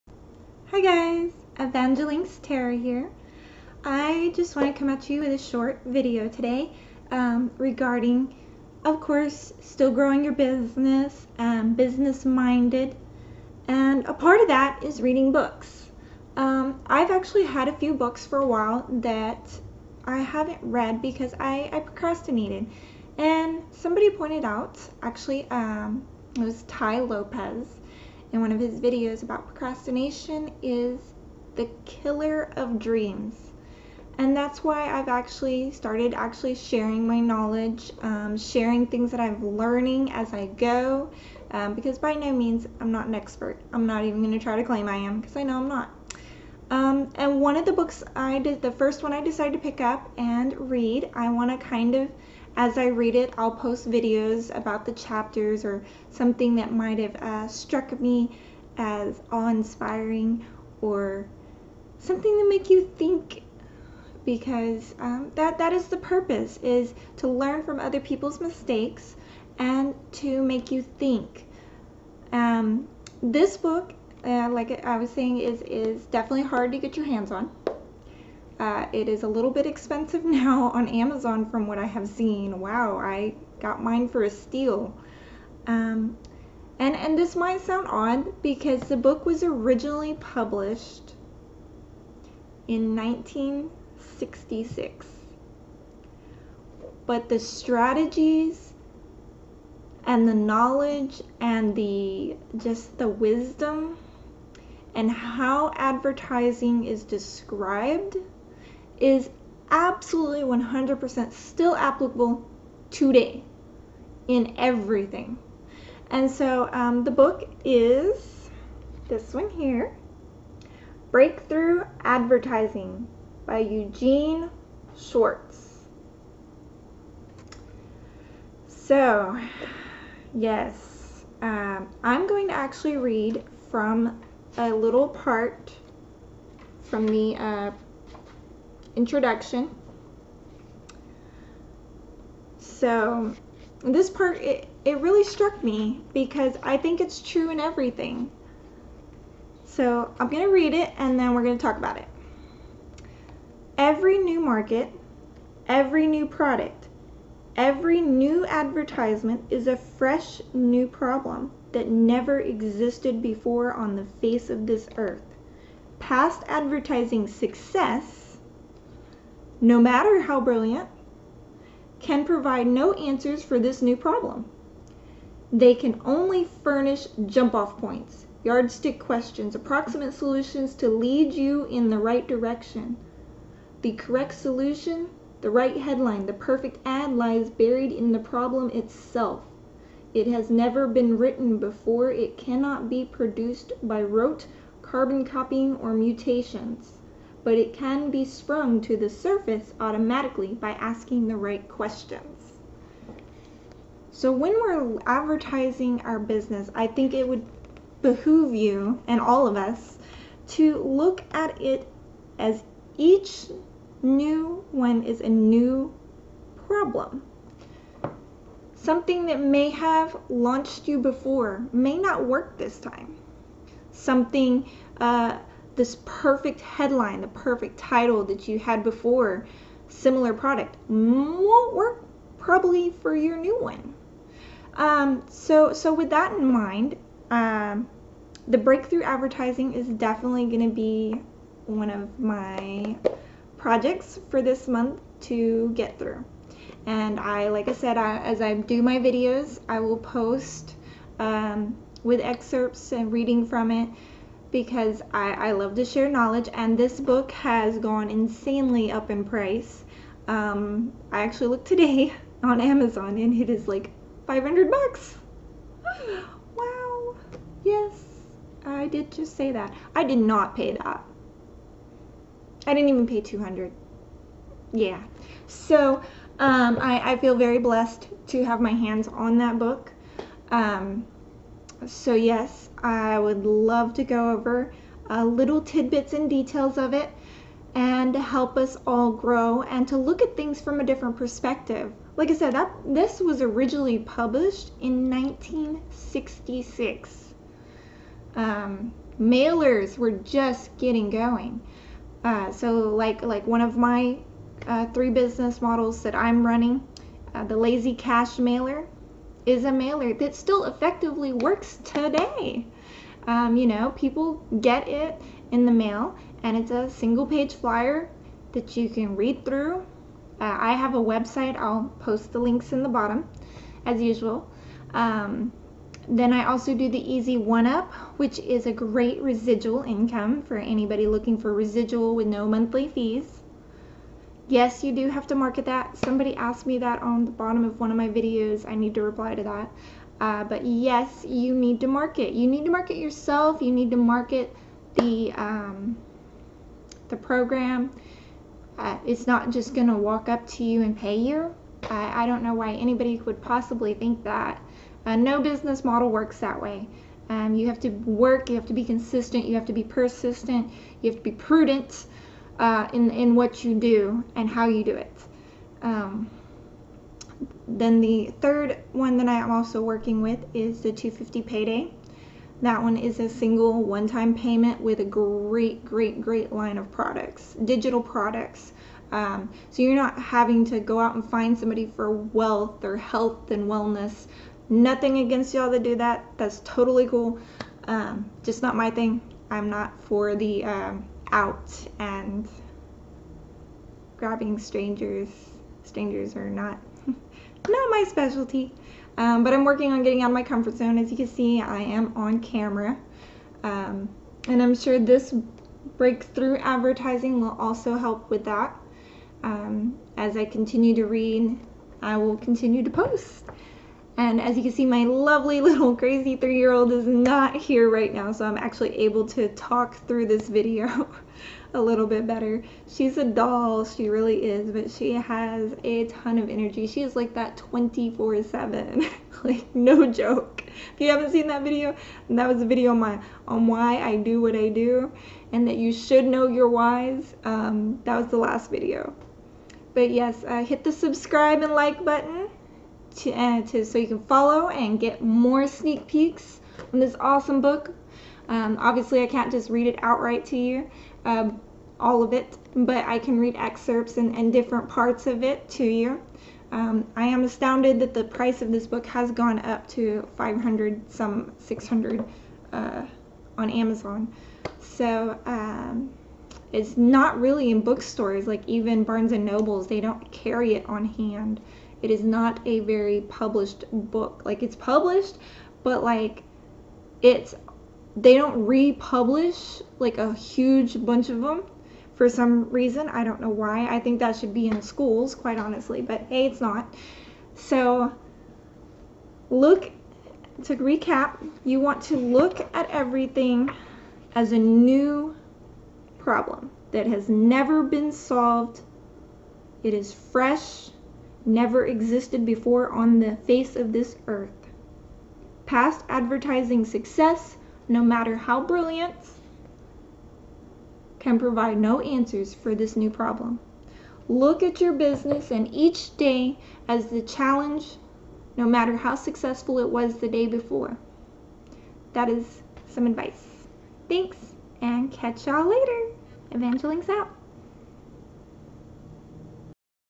Breakthrough Advertising Eugene Schwartz
Breakthrough Advertising Eugene Schwartz for your business originally published in 1966. Astounding applications in today's' business models.

Book for April, going to try to read a book a month and share excerpts of them as I go.